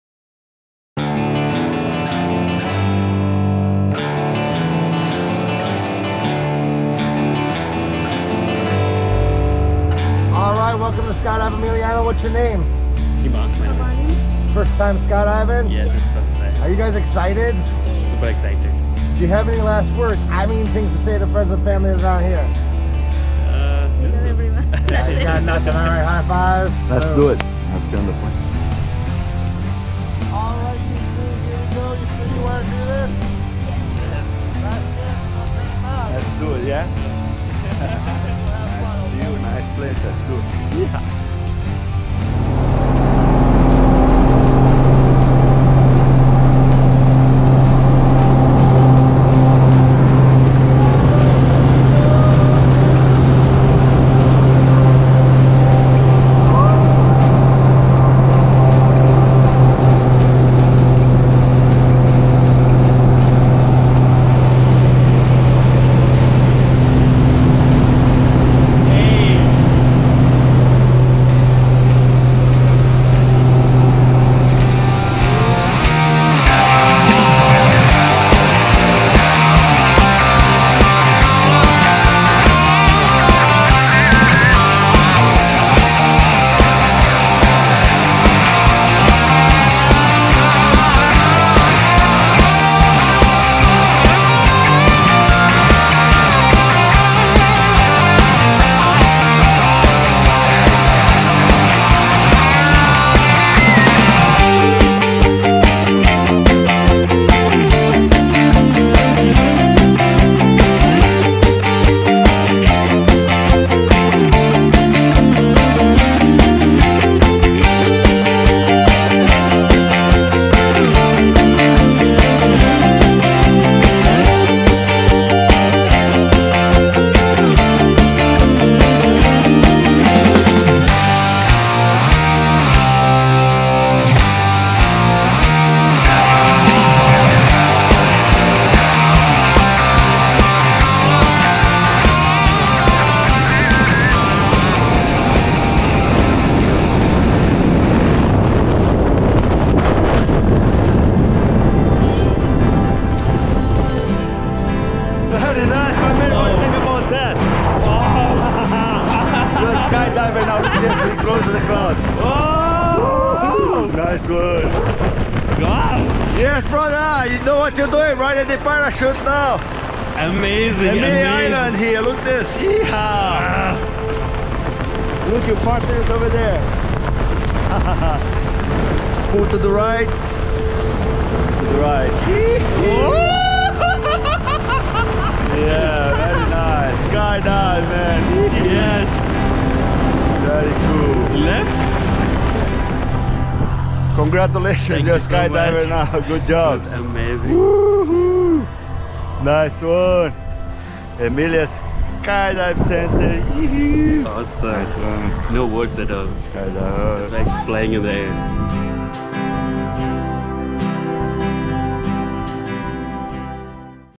Hima - Skydive Amelia Island
Skydive over the beach at Amelia Island. Tandem Skydive near Jacksonville Florida - Hima